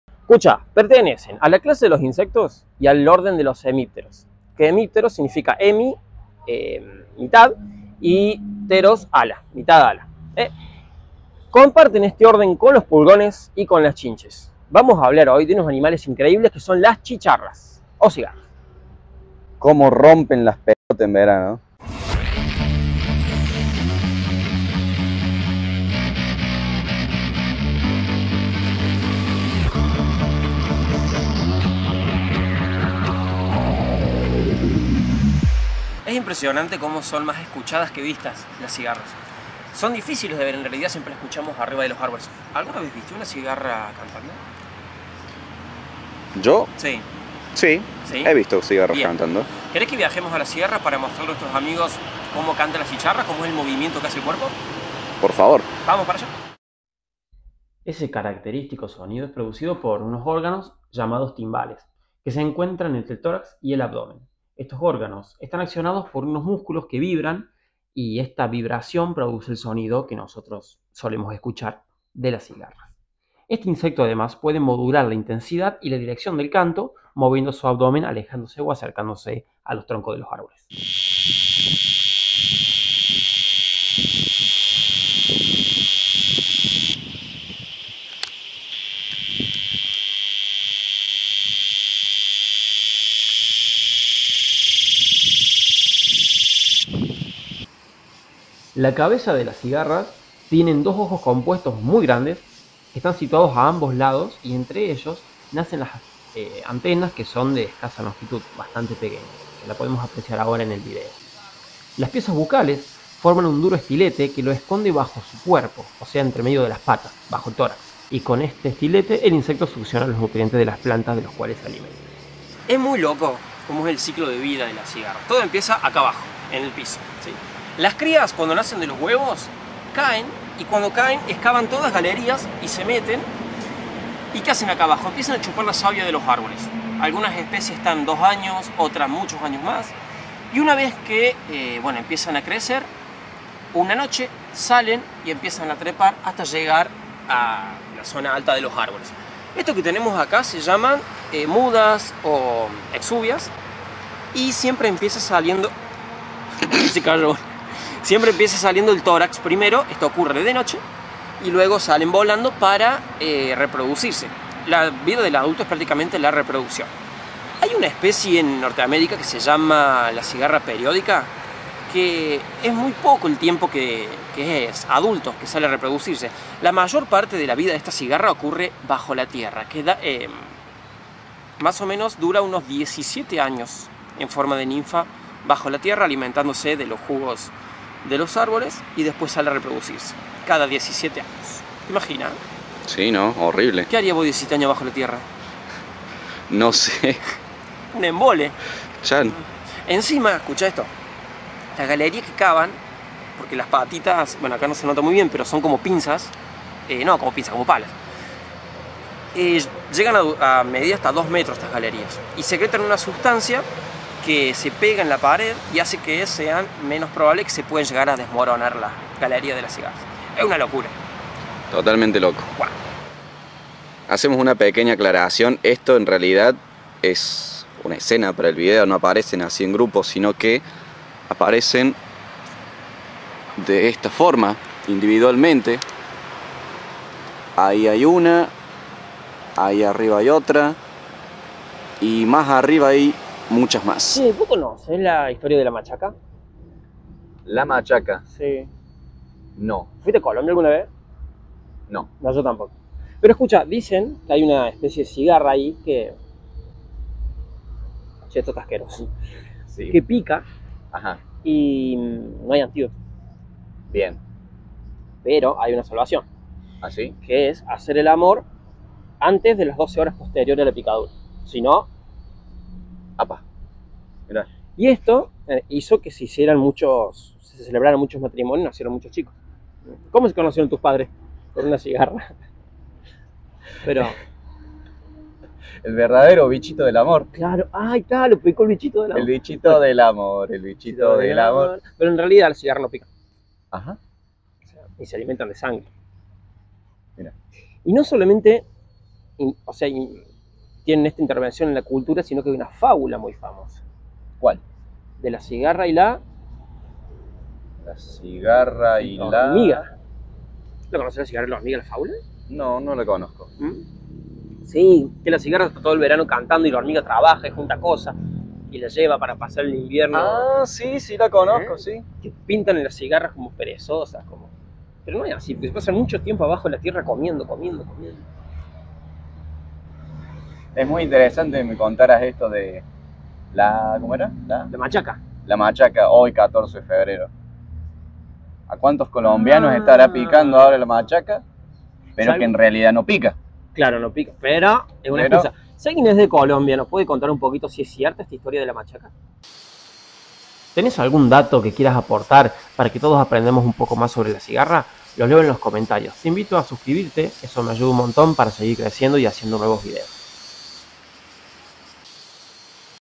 Cigarra al canto
Las cigarras son insectos del orden de los hemípteros con una vida muy curiosa, te invitamos a conocer un poco de su biología.